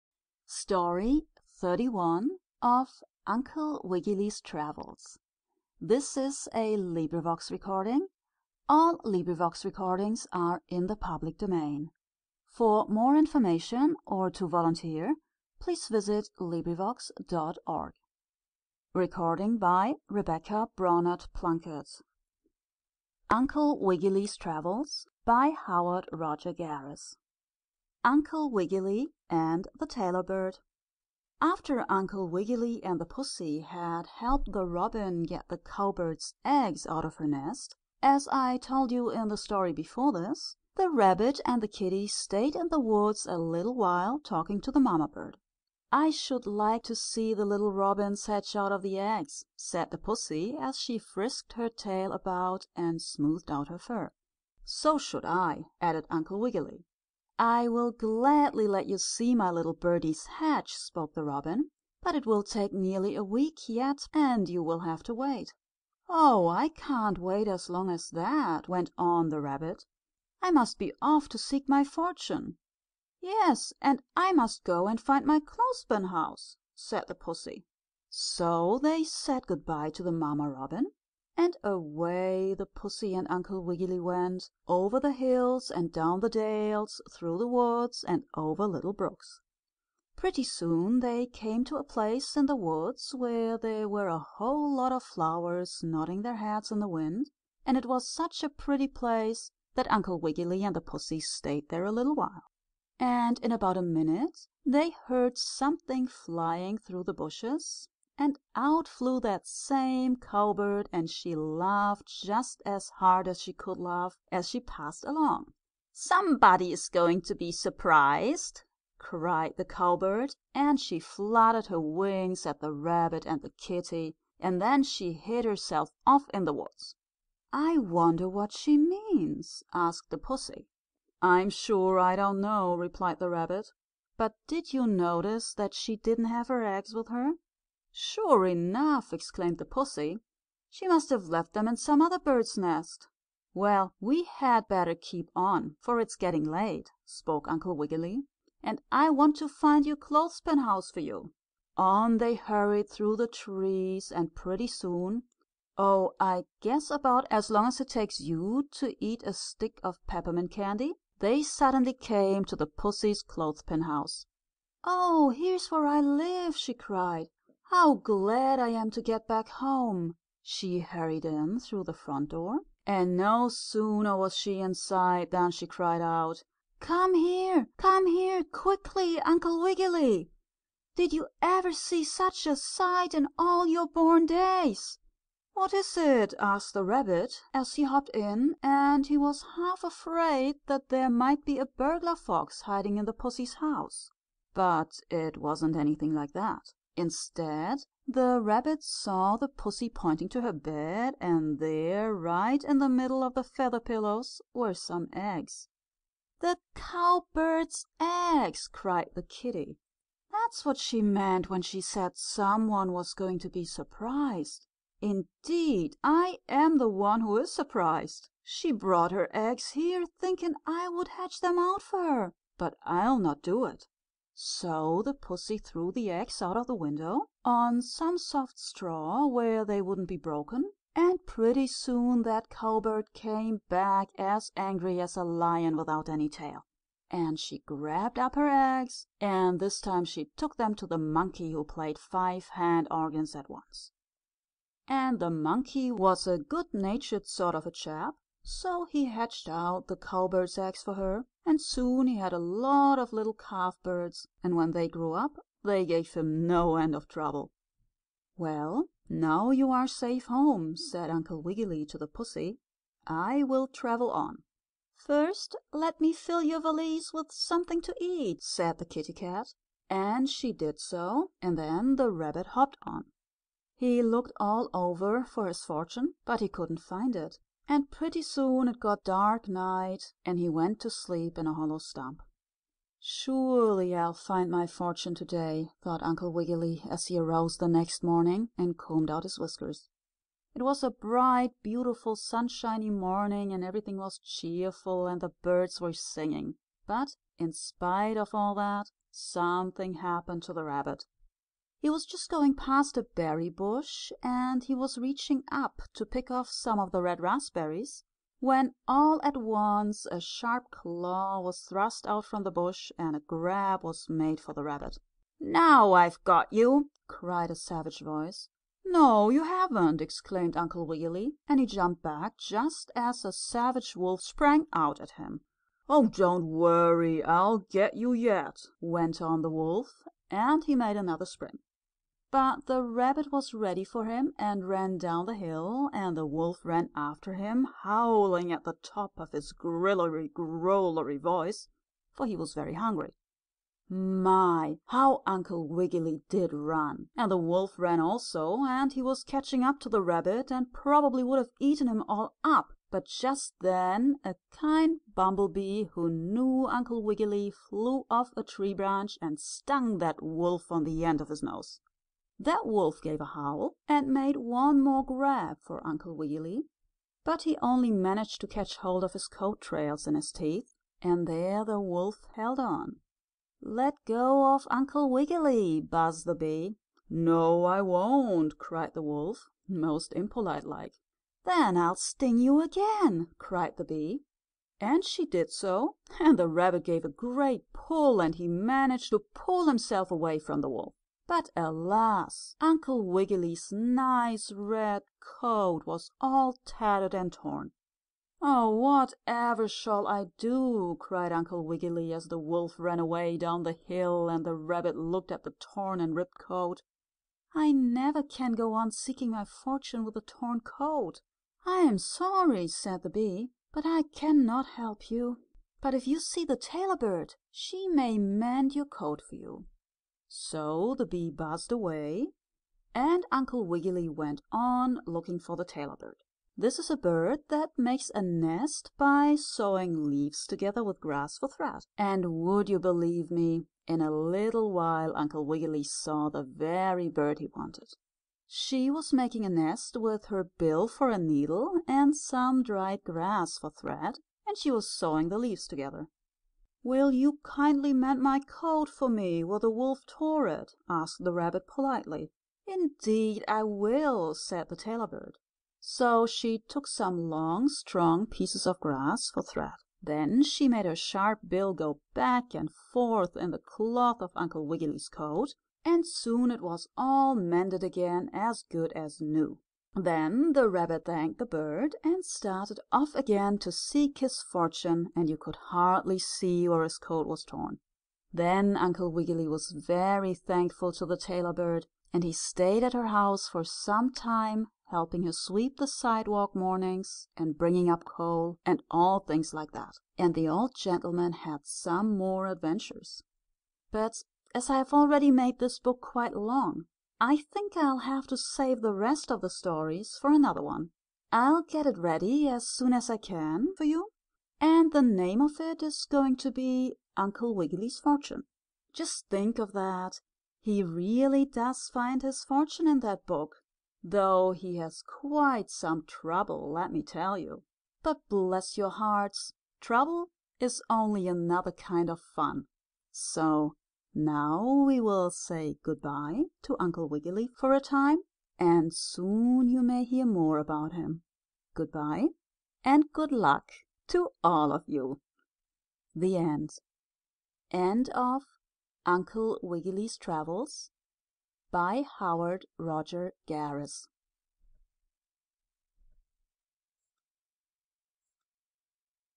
Uncle Wiggily's Travels Chapter 31: Uncle Wiggily and the Tailor Bird
Title: Uncle Wiggily's Travels
Chapter 31: Uncle Wiggily and the Tailor Bird
Author: Howard R. Garis
Performer: LibriVox - Rebecca Braunert-Plunkett